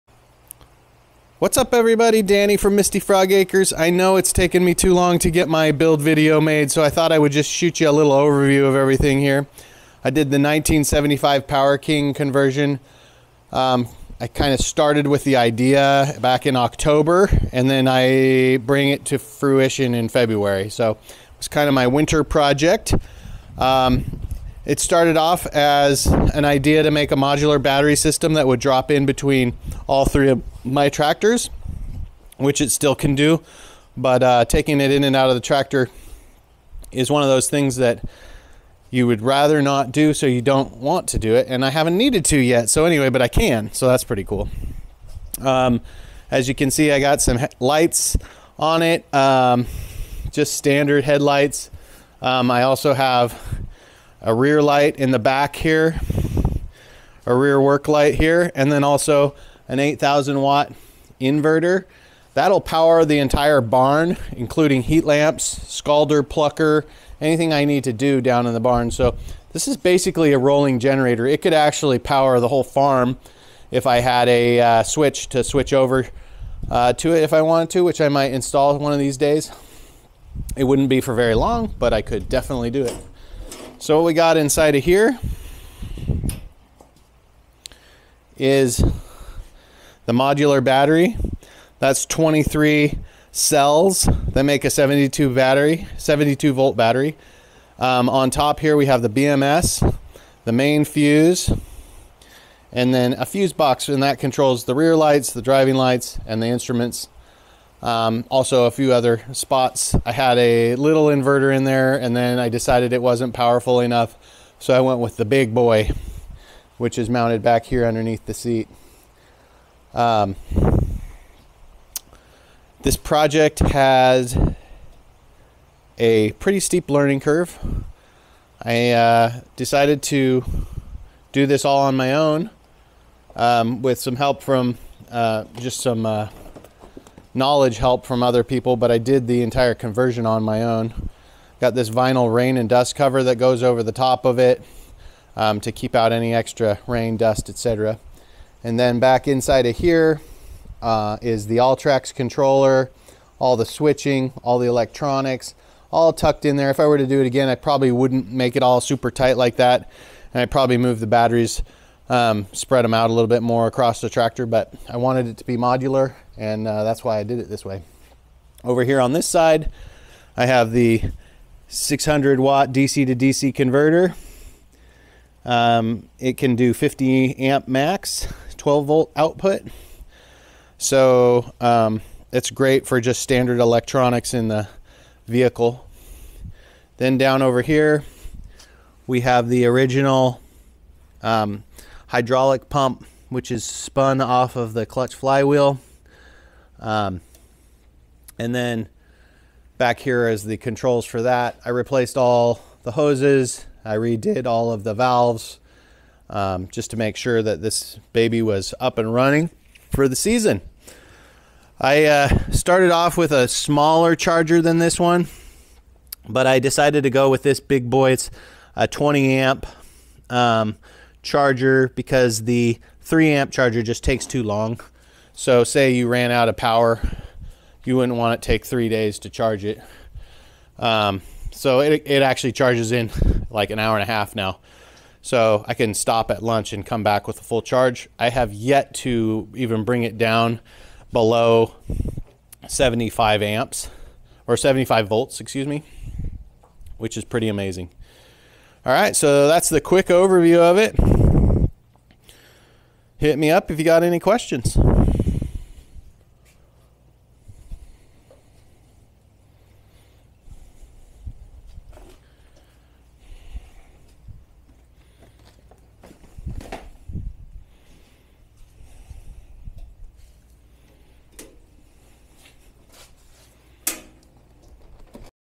EV tractor conversion overview
Danny does a quick overview of Big AL, the newest addition to the farm.  Danny took the Winter to convert his old 1975 Power King into a fully battery electric vehicle.  It has enough power to work all day at the pastured poultry operation, that includes pulling chicken tractors, and regular farm chores.  Here is a quick overview of the machine!